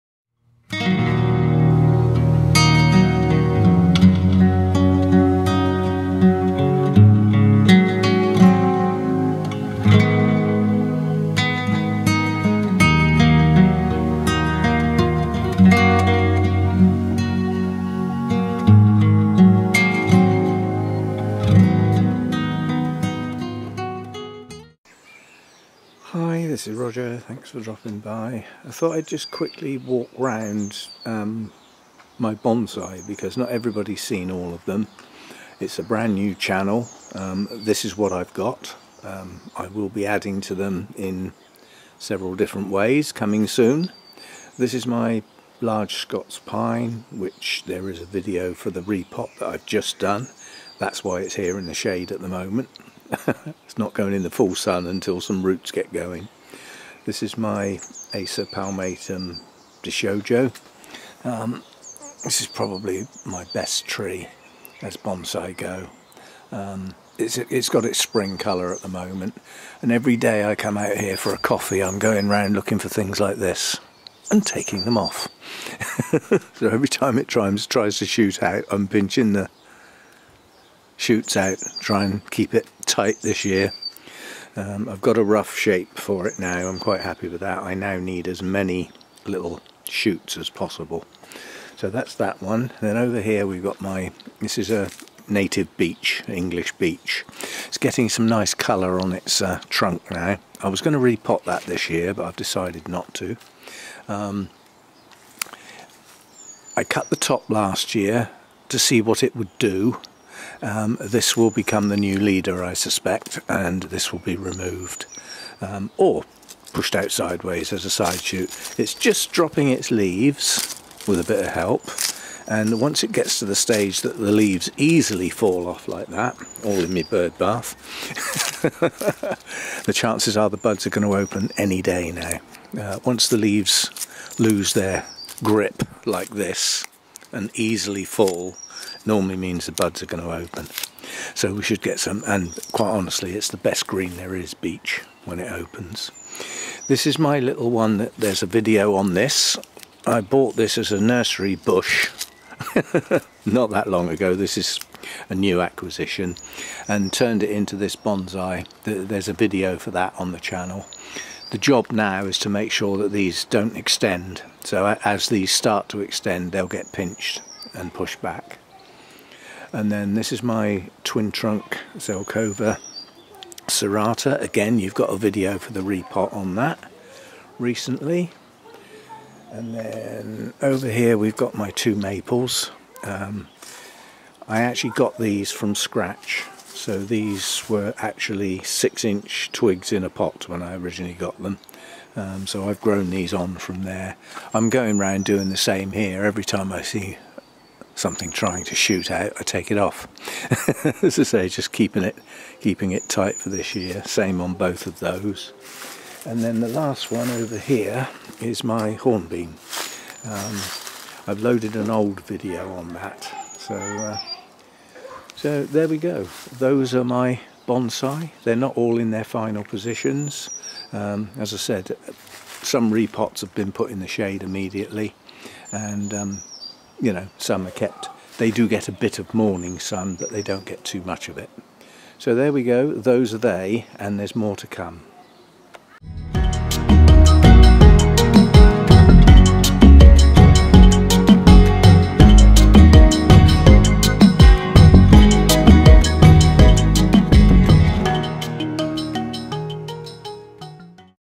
A Quick Look at All My Bonsai Outside Around the Garden
This is all the Bonsai I have at the moment in mid-April 2022. I have plans to collect at least one more tree from the wild to train over the years and I've bought some young trees to start from scratch. Videos will follow on these soon. At the end is a link to my Bonsai as they were in July 2019.
(Initially, videos have been moved across from my Orchid Channel "Roger's Orchids". In future, new Bonsai Videos will only be loaded here.)